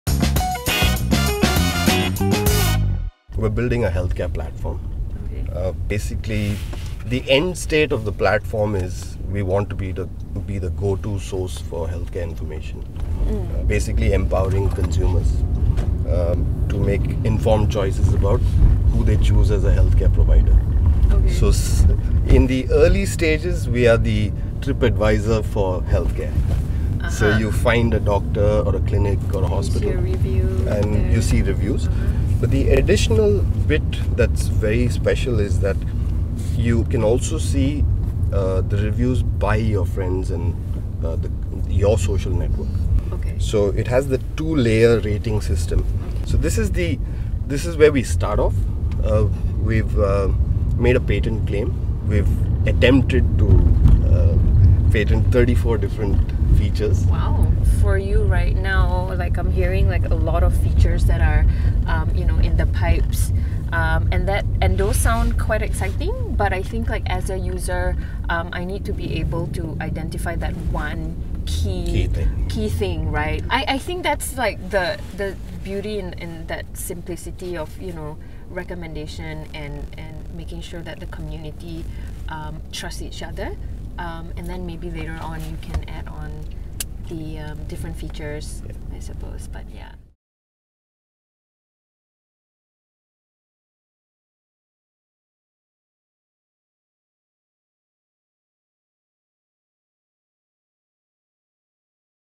Arun Augustine Pitches To Judith Yeoh (SAYS Head of Content)
IDEA: Helping consumers 'Find Good Health'.